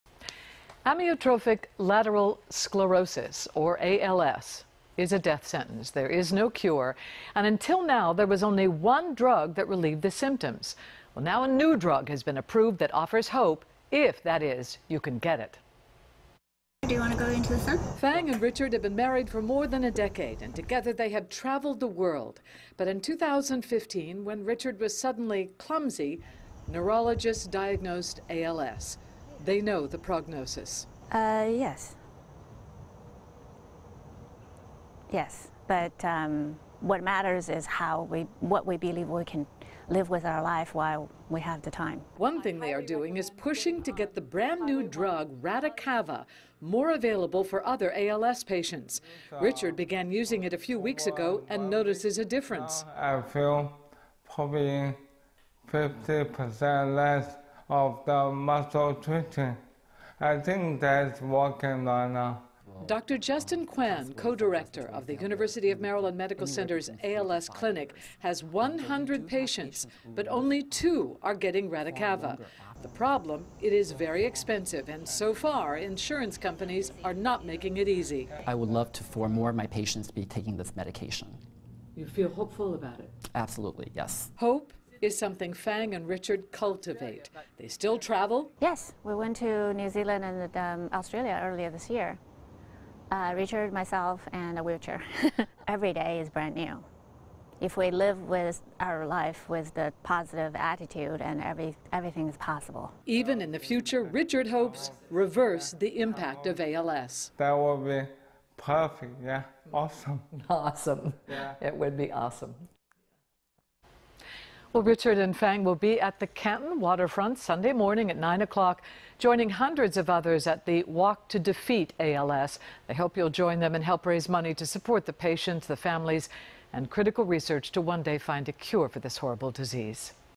New Drug Gives ALS Patients Hope
Patients with ALS, amyotrophic lateral sclerosis, go through what is often known as a death sentence. There is a new drug called Radicava available that offers some hope.